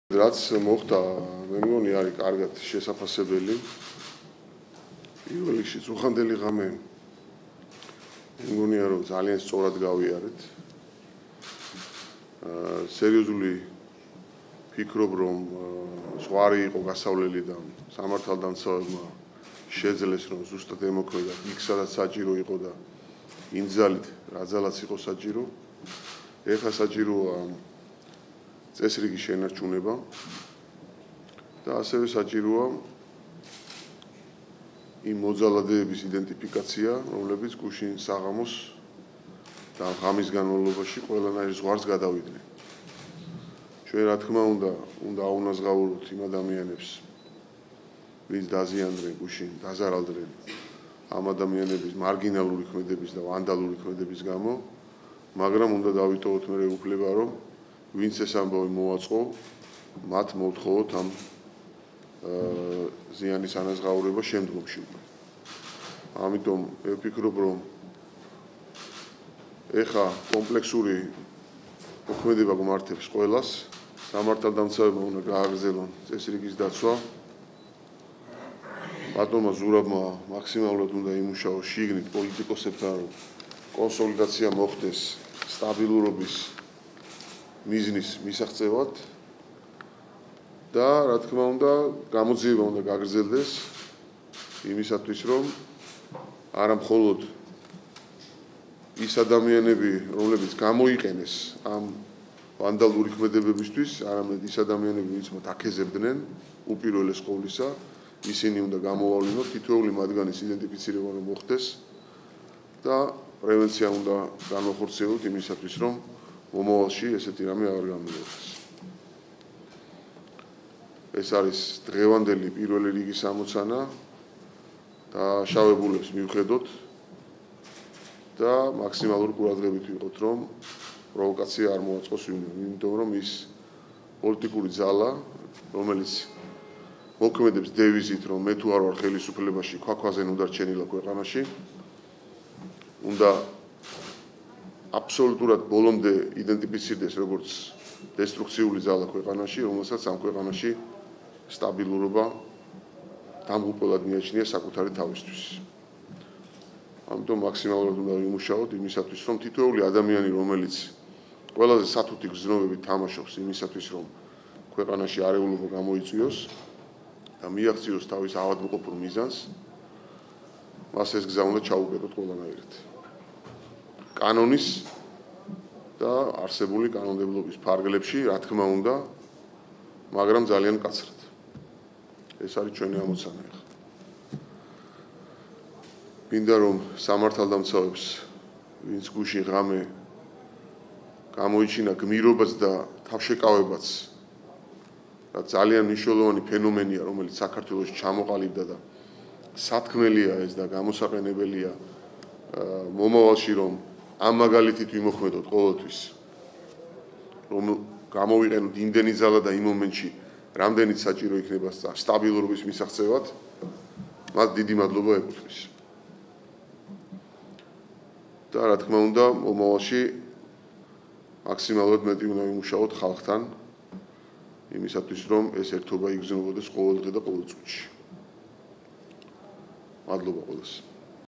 პრემიერი - წუხელ გასავლელი იყო ძალიან სერიოზული ზღვარი, რაც ხელისუფლებამ ძალიან სწორად გაიარა
წუხანდელი ღამის განმავლობაში გასავლელი იყო ძალიან სერიოზული ზღვარი, რაც ხელისუფლებამ ძალიან სწორად გაიარა, - ამ სიტყვებით დაიწყო საქართველოს პრემიერ-მინისტრმა თათბირი აჭარის ავტონომიური რესპუბლიკის მთავრობაში.
გიორგი კვირიკაშვილის განცხადებით, სამართალდამცავებმა შეძლეს, ზუსტად ემოქმედათ იქ, სადაც და რა ძალითაც იყო საჭირო; ახლა კი საჭიროა წესრიგის შენარჩუნება და იმ მოძალადეების იდენტიფიკაცია, რომლებიც გუშინ "ყველანაირ ზღვარს გადავიდნენ".
"ჩვენ, რა თქმა უნდა, ზარალი უნდა ავუნაზღაუროთ მათ, ვინც ამ ადამიანების მარგინალური და ვანდალური ქმედებების გამო დაზარალდა, მაგრამ უნდა დავიტოვოთ უფლება, რომ ვინც ეს ამბავი მოაწყო, მათ მოვთხოვოთ ამ ზიანის ანაზღაურება შემდგომში“, - აღნიშნა პრემიერ-მინისტრმა.
მთავრობის მეთაური მიიჩნევს, რომ ახლა საჭიროა კომპლექსური მოქმედება და კონსოლიდაცია სტაბილურობის მიზნის მისაღწევად, ასევე, უნდა გაგრძელდეს გამოძიება, რათა არა მხოლოდ ვანდალური ქმედებებისთვის გამოყენებული ადამიანები დაისაჯონ, არამედ გამოვლინდნენ წამქეზებლები დამოხდეს პრევენცია რომ მსგავსი რამ აღარ განმეორდეს.
„ეს არის დღევანდელი პირველი რიგის ამოცანა, დაშავებულებს მივხედოთ და მაქსიმალური ყურადღებით ვიყოთ, რომ პროვოკაცია არ მოაწყოს ვინმემ, რადგან ის პოლიტიკური ძალა, რომელიც მოქმედებს დევიზით, მე თუ არ ვარ ხელისუფლებაში, ქვა ქვაზე ნუ დარჩენილა ქვეყანაში, აბსოლუტურად ბოლომდე უნდა იდენტიფიცირდეს, როგორც დესტრუქციული ძალა, რომელსაც ამ ქვეყანაში სტაბილურობა დამღუპველად მიაჩნია საკუთარი თავისთვის. ამიტომ, მაქსიმალურად უნდა ვიმუშაოთ იმისთვის, რათა თითოეულ ადამიანს, რომელიც ყველაზე სათუთი გრძნობებით თამაშობს, რათა ქვეყანაში არეულობა გამოიწვიოს და მიაღწიოს თავის ავადმყოფურ მიზანს, ეს გზა ჩავუკეტოთ არსებული კანონმდებლობის ფარგლებში, მაგრამ ძალიან მკაცრად. ეს არის ჩვენი ამოცანა“, - განაცხადა პრემიერ-მინისტრმა.
გიორგი კვირიკაშვილმა კიდევ ერთხელ განსაკუთრებული მადლობა გადაუხადა სამართალდამცავებს, რომლებმაც გასული ღამის განმავლობაში გმირობა და თავშეკავება გამოიჩინეს.
პრემიერის შეფასებით, ჩამოყალიბდა ძალიან მნიშვნელოვანი ფენომენი საქართველოში და მომავალშიც, ხელისუფლებამ უნდა იმოქმედოს ამავე მაგალითით - გამოიყენოს იმდენი ძალა, რამდენიც კონკრეტულ მომენტშია საჭირო სტაბილურობის მისაღწევად.
„რა თქმა უნდა, მომავალში მაქსიმალურად მეტი უნდა ვიმუშაოთ ხალხთან, იმისათვის, რომ ეს ერთობა იგრძნობოდეს ყოველდღე და ყოველ წუთში“, - აღნიშნა პრემიერმა.